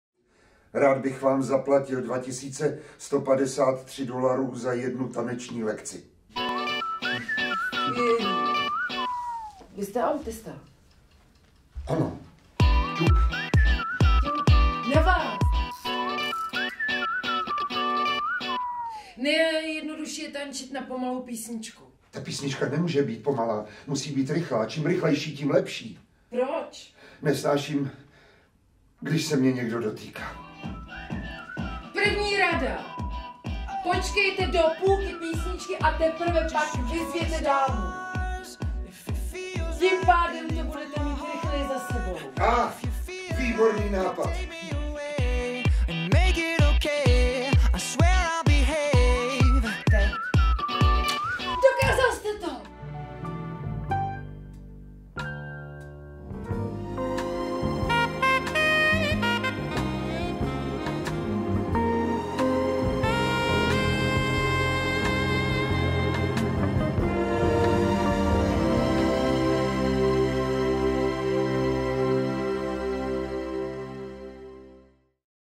Trailer | Taneční hodiny
Trailer - TANEČNÍ HODINY - Mark St. Germain

Jak naučit tančit autistického profesora, který nemá rád doteky? Stačí ho poslat k profesionální tanečnici. Co na tom, že má nohu v ortéze a sotva chodí... Romantická komedie o sblížení dvou samotářů, jejichž silná vůle možná překoná i nepřekonatelné.

Hrají Miroslav Táborský a Petra Nesvačilová. Režie Pavel Ondruch.